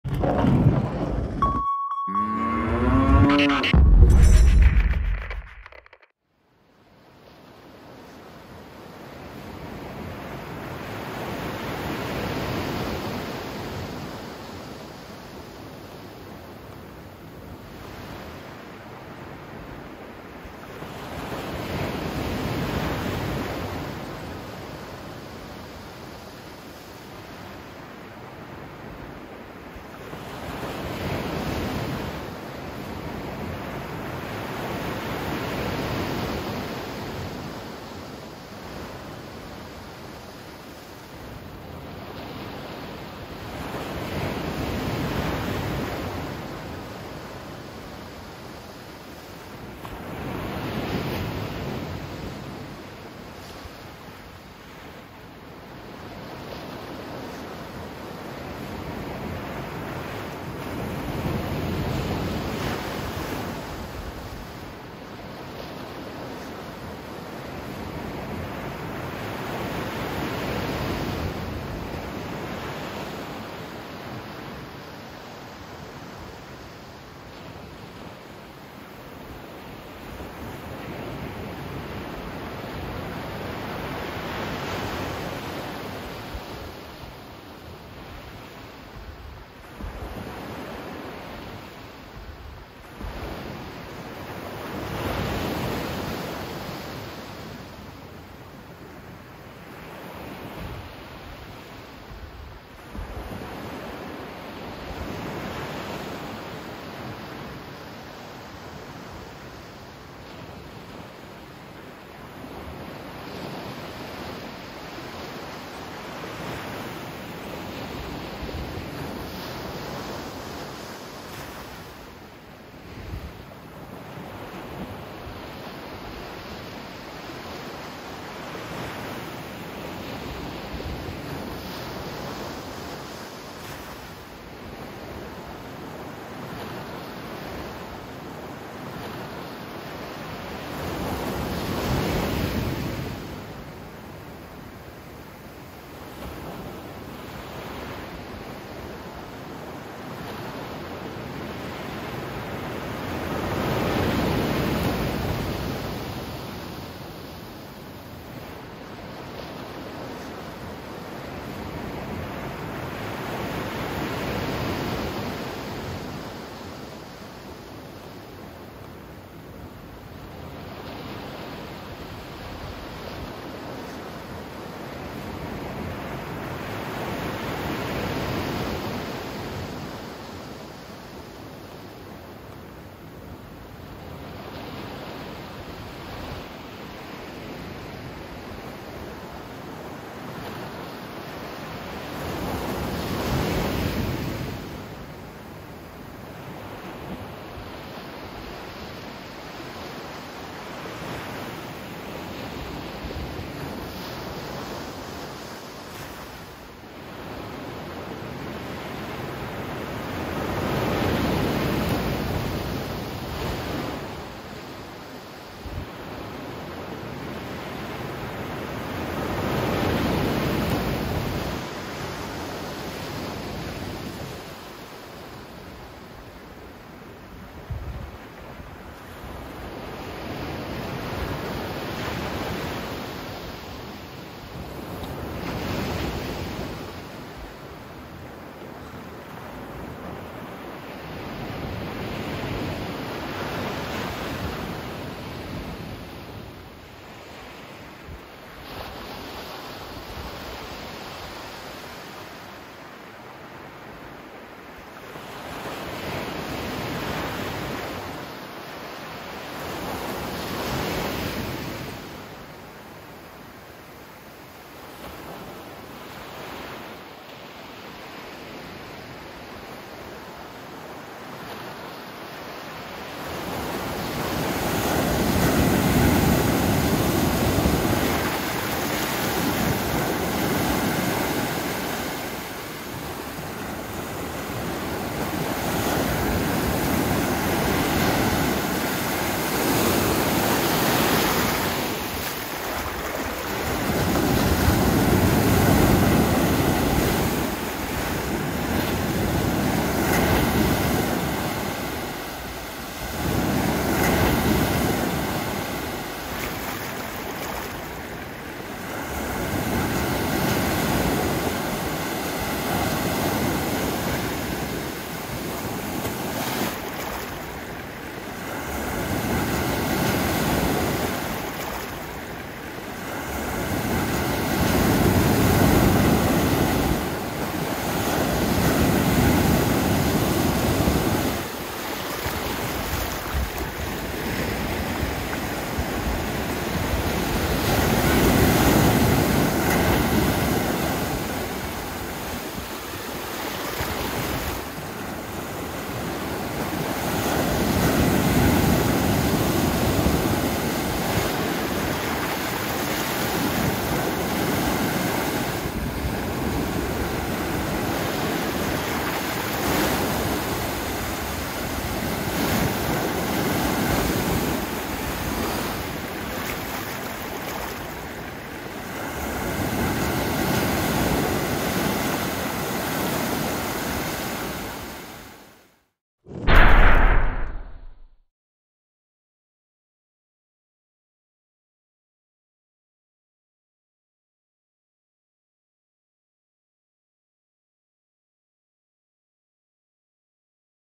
Sounds of Ocean Waves on the Beach | Tropical Island Ambience w/FREE Sound Effects and Stock Video
FREE sounds and video of gentle ocean waves washing ashore on a tropical beach. Waves crashing against lava rocks along a cliffside.

Wide angle, medium range and close up shots from a tropical Hawaiian island. Beautifully clear, turquoise ocean water.

Index Markers:
00:08 wide shot of tropical beach and lava rock
04:08 medium shot of gentle waves washing up on sand
04:35 close up overhead view of multi directional waves crossing paths and crashing along the sand